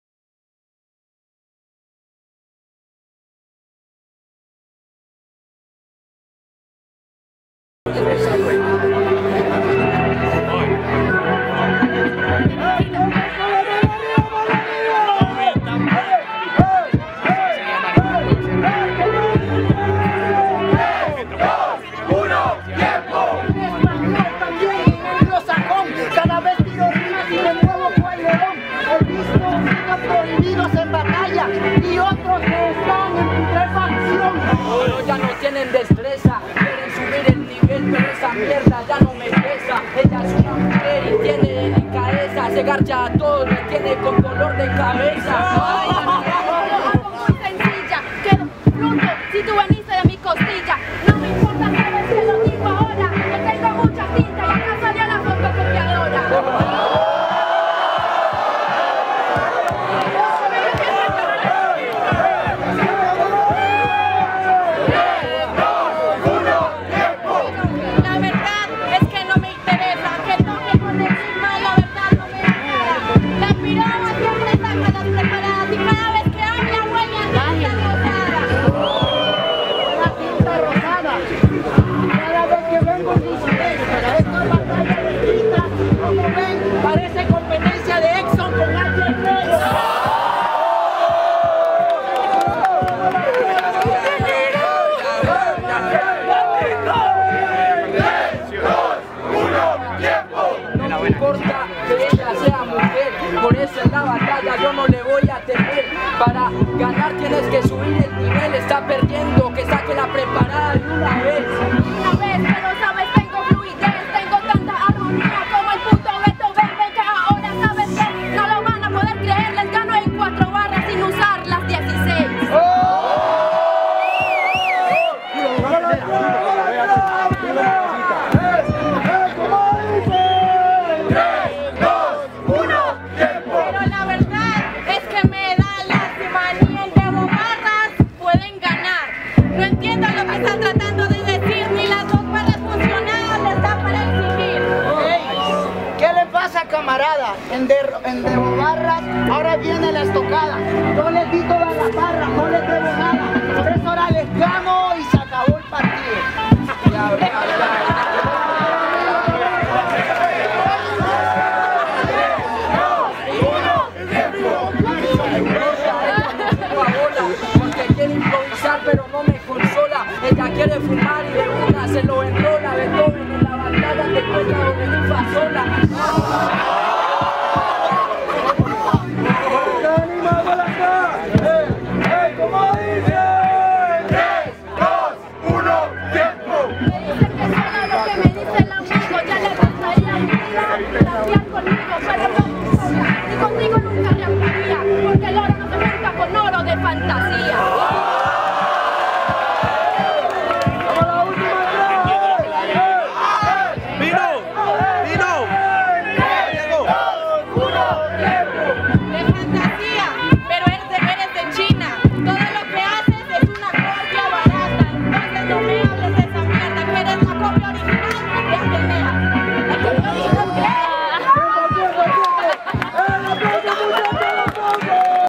ENIGMA vs MENEZ vs LUISSEN vs | Filtro Gold Battle Medellin
Primera clasificatoria para la final regional de Gold Battle Medellin 2019.

Recuerda cada 15 días estamos en Parques del Rio (Medellin) realizando batallas de Freestyle.